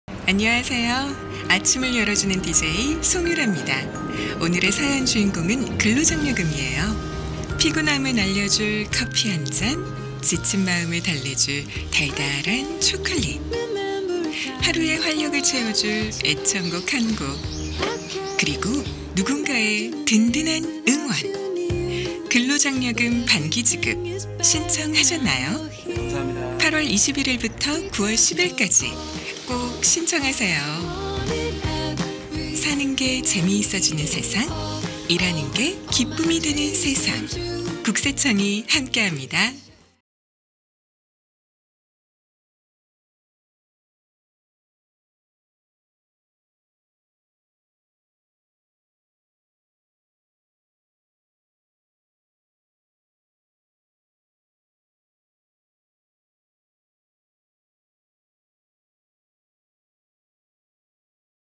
국세청 근로장려금 반기신청제도
국세청이 알려드리는 근로장려금 반기신청제도!
꼼꼼하고 똑똑하게 챙겨 보다 큰 혜택을 누리세요!

그리고 국세청의 여러 영상도 함께 보시면 좋다는거~👍